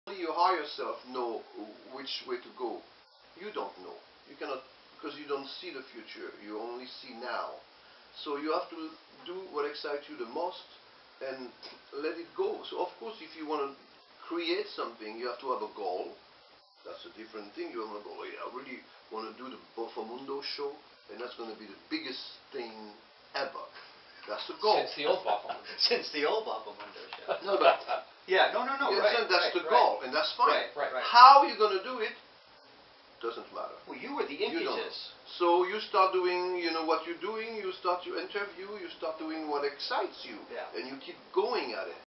Wise Words From Bunny Brunel
Bassman extraordinaire, Bunny Brunel, inspires Aaron and Ron about the New Boffomundo Show and for everyone.  New show coming soon!